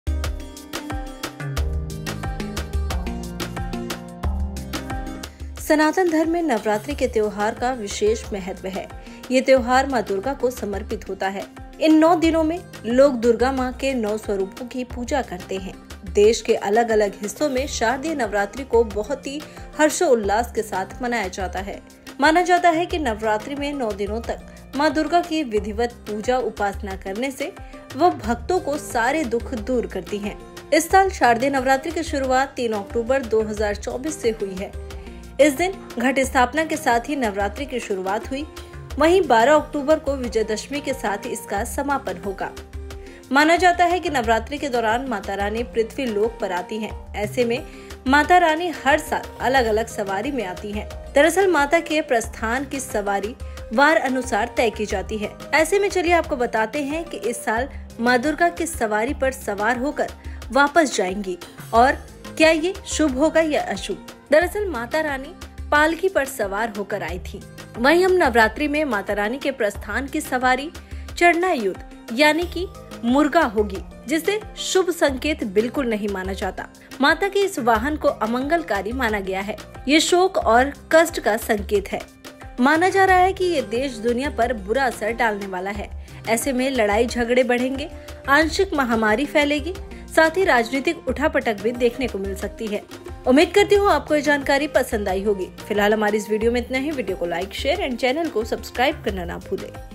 Maa Durga Ki Sawari 2024: मुर्गे पर सवार होकर विदा होंगी मां दुर्गा, इसका असर शुभ या अशुभ | Boldsky
माता रानी हर साल अलग-अलग सवारी में आती हैं। दरअसल, माता के प्रस्थान की सवारी वार अनुसार तय की जाती है। ऐसे में आइए जानते हैं इस साल मां दुर्गा किस पर सवार प्रस्थान करेंगी और इसका क्या असर होगा.

Every year, Mata Rani arrives in a different ride. Actually, the ride of Mata's departure is decided according to the day. So let's know on which ride Maa Durga will depart this year and what will be its effect.